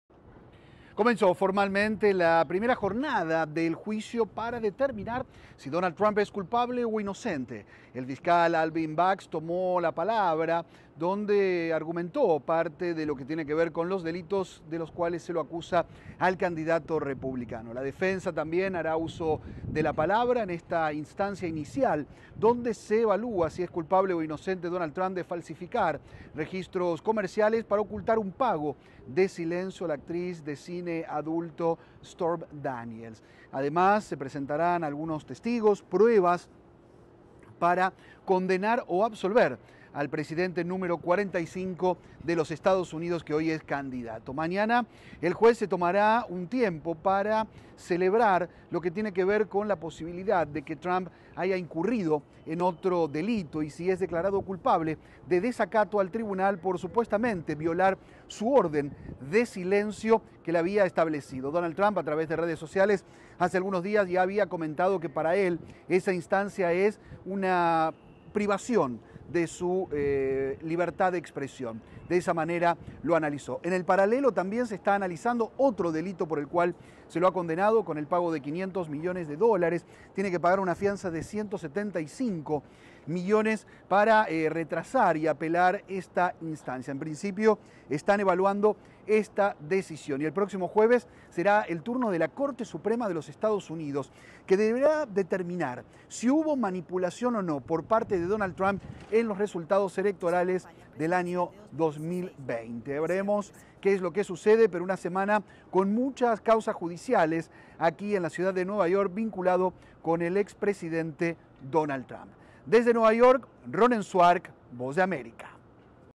Inicia la primera jornada del juicio contra Donald Trump
Los alegatos iniciales comenzarán hoy lunes en el juicio de Donald Trump en Nueva York, el primer caso penal jamás iniciado contra un expresidente de Estados Unidos.